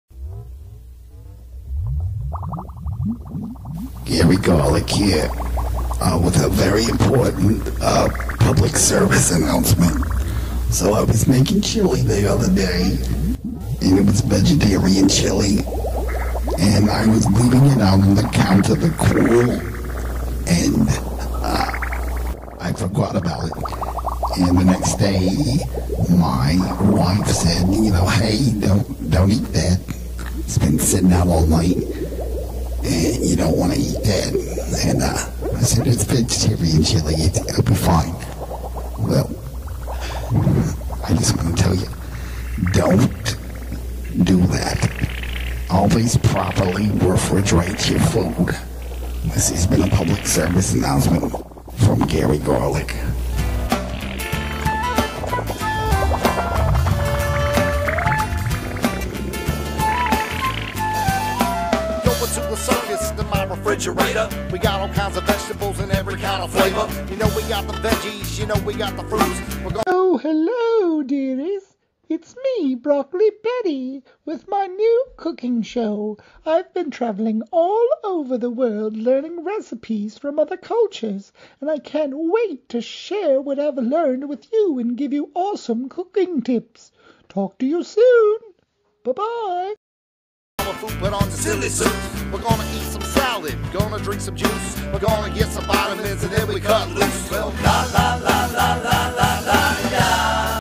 Gary Garlic PSA 1
quick little public service announcement from Gary Garlic host of soup segment of Broccoli Betty's new cooking show